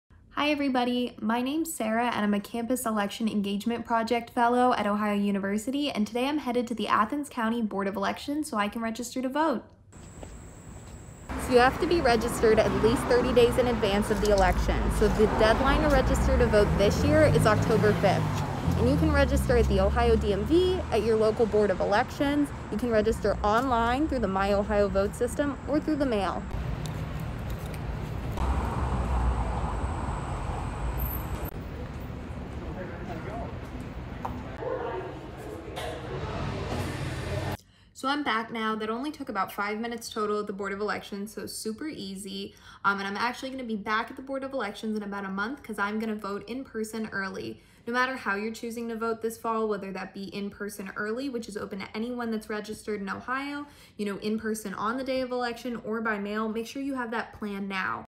Bobcats Register to Vote!
Sarah Donaldson, a Campus Election Engagement Project Fellow at the Ohio University Center for Campus and Community Engagement, shares her experience registering to vote.